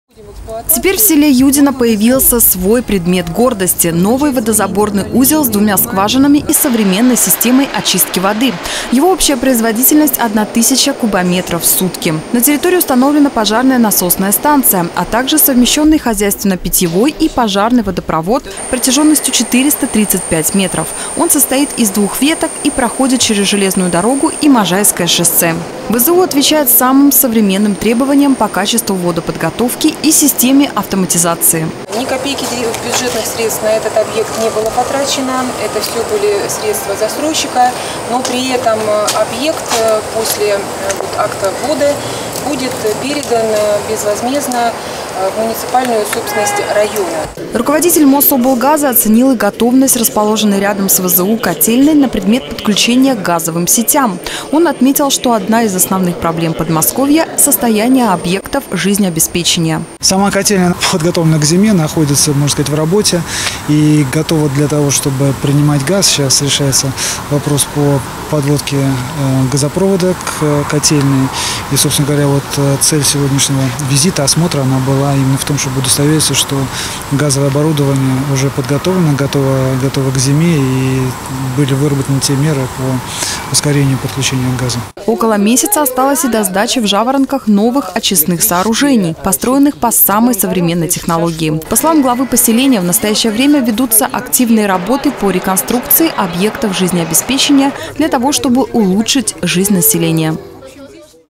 Открытие нового ВЗУ в Юдино
В Юдино состоялось торжественное открытие нового водозаборного узла. Он позволит обеспечить жителей Жаворонковского поселения качественной питьевой водой, соответствующей всем санитарным нормам. Кроме того ВЗУ будет питать водой гипермаркеты «Леруа Мерлен», «Глобус», торговый комплекс «Леденцово – Парк» и перспективную застройку.